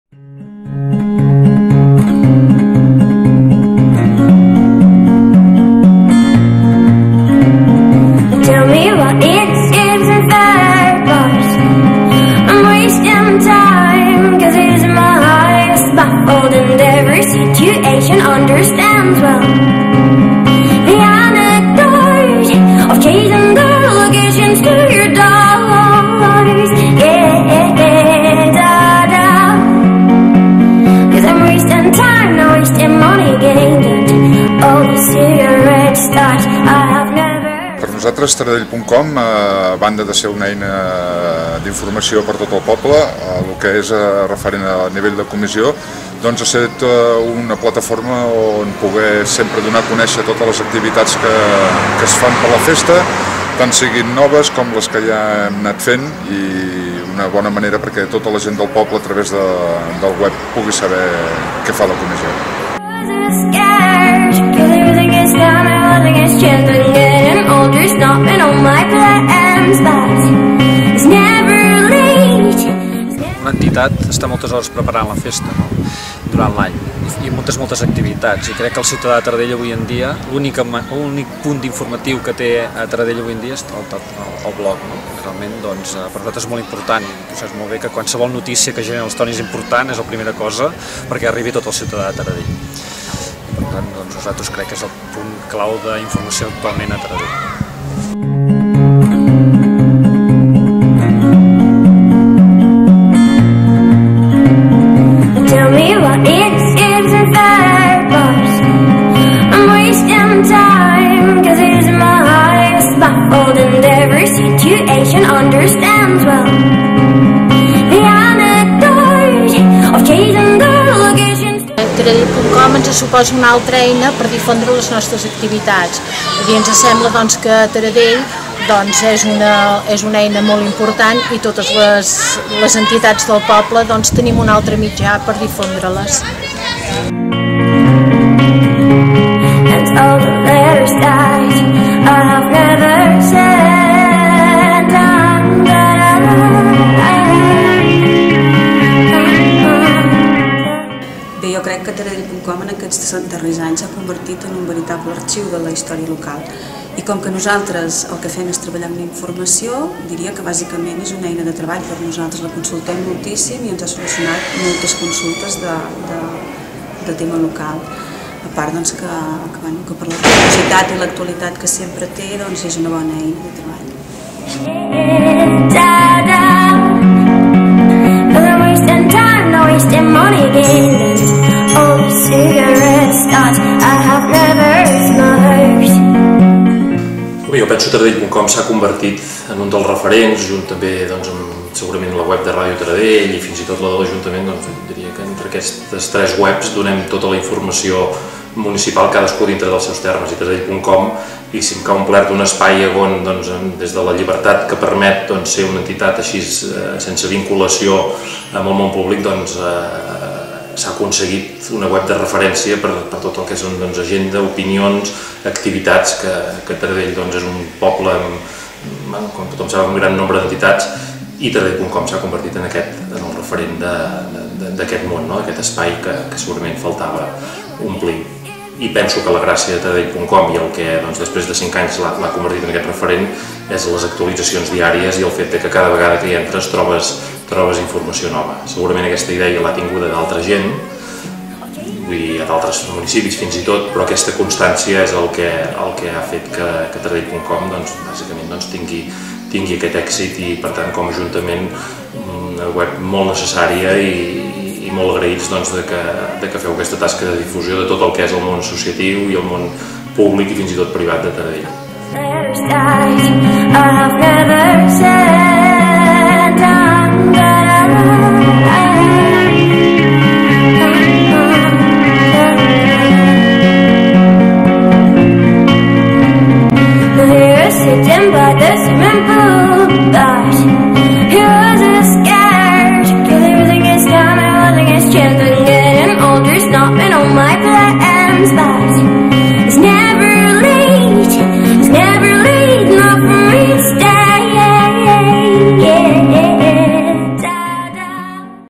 5 anys de Taradell.com
Petit muntatge audiovisual per commemorar els 5 anys de la creació del blog de Taradell i per celebrar el canvi d'imatge i la renovació de la web